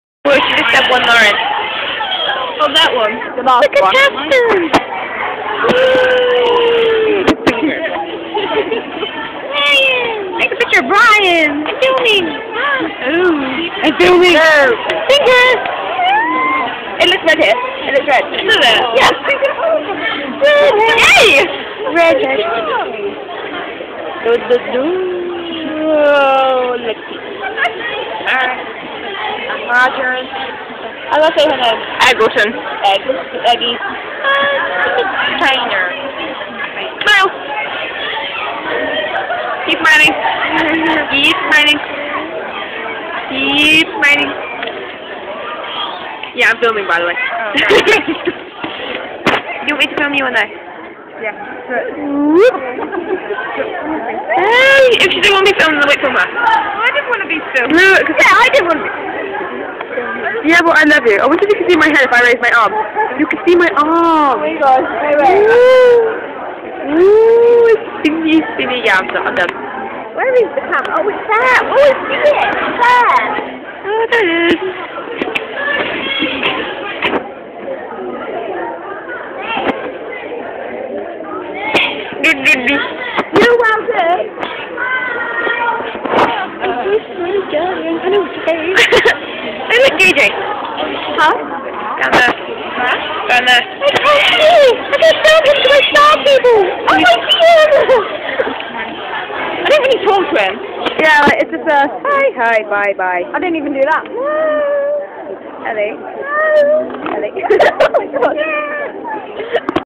Coombes Factor randomness1
Just meh and Ellie messing around during the show :D